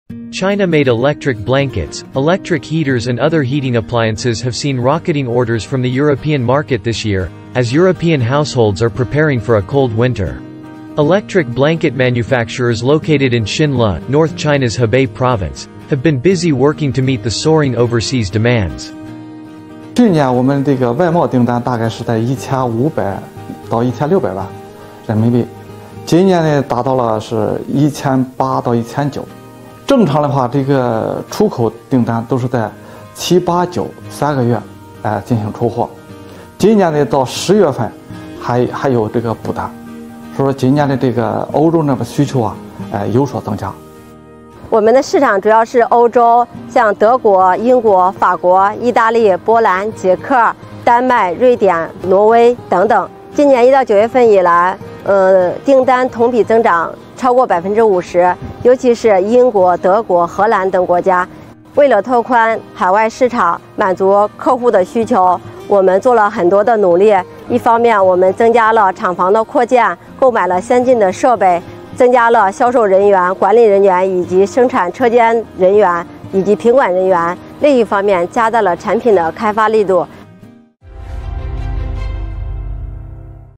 GLOBALink | Chinese heating appliance manufacturers see surging orders from Europe
China-made heating appliances have seen rocketing orders from the European market this year as European households are preparing for a cold winter.